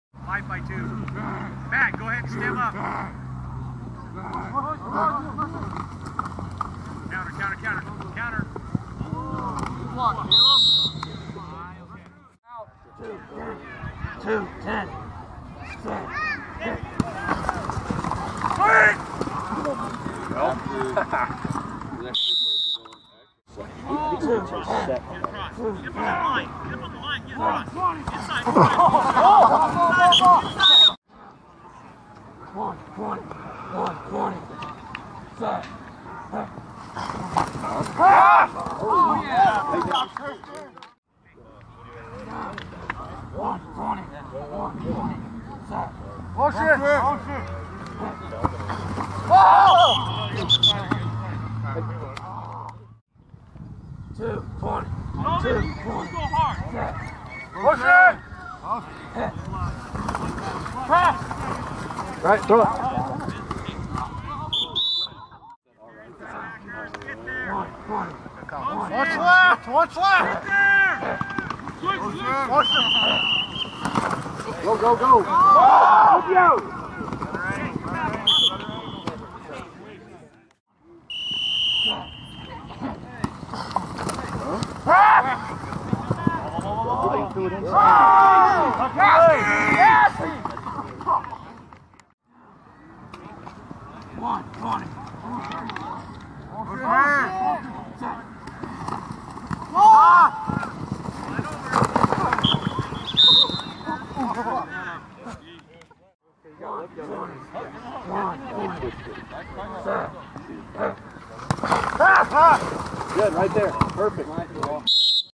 RHS ScrimmageHD
Sights and sounds from the Riverton Wolverine football scrimmage Saturday, Aug. 23, 2014 at Wolverine Field. (You may enjoy about 25 seconds into the video). Go Big Red!